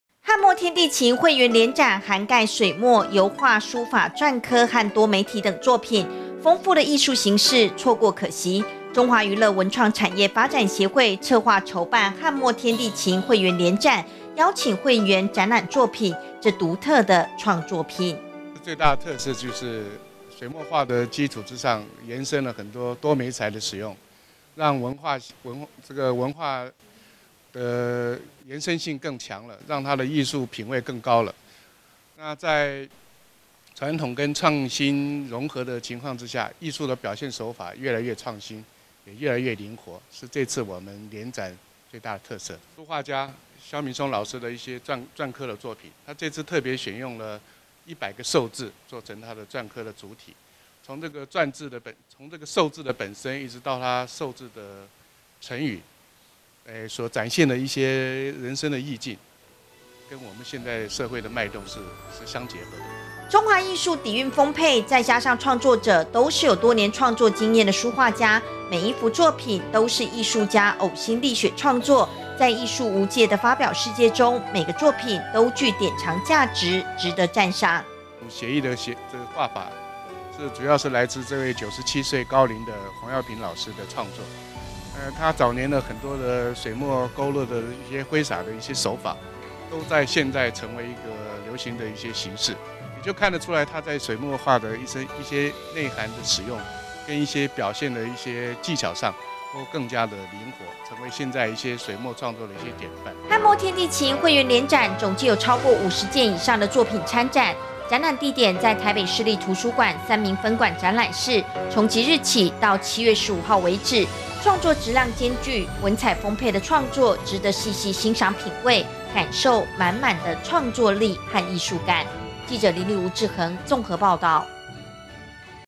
1090707 【中嘉】 翰墨天地情 會員聯展作品獨一無二
傳承中華文化精髓，推廣文化藝術活動，中華娛樂文創產業發展協會策劃舉辦「翰墨天地情」會員聯展，展示最新書畫作品超過50件，備受藝文界肯定。